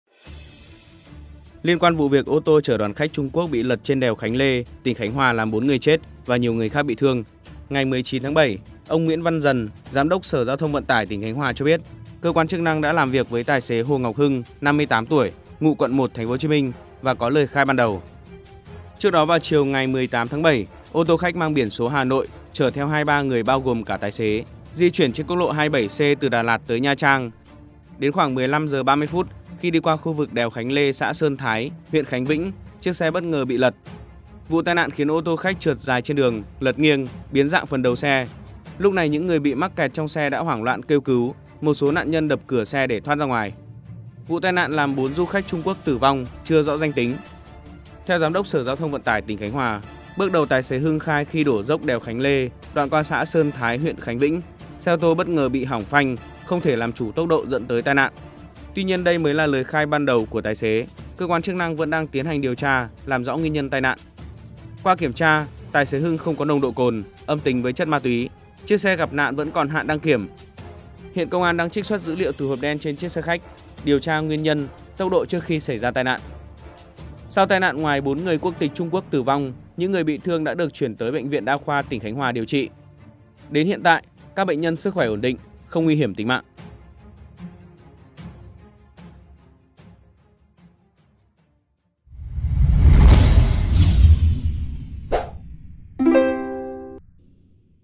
Tài xế ô tô chở đoàn khách Trung Quốc gặp n.ạ.n ở Khánh Hòa khai gì? | VTC14
VTC14 | Liên quan vụ việc ô tô chở đoàn khách Trung Quốc b.ị l.ậ.t trên đèo Khánh Lê, tỉnh Khánh Hòa làm 4 người c.h.ế.t và nhiều người khác b.ị t.h.ư.ơ.ng. Ngày 19/7, ông Nguyễn Văn Dần, Giám đốc Sở GTVT tỉnh Khánh Hòa cho biết, cơ quan chức năng đã làm việc với tài xế Hồ Ngọc Hưng (58 tuổi, ngụ quận 1, TP.HCM) và có lời khai ban đầu.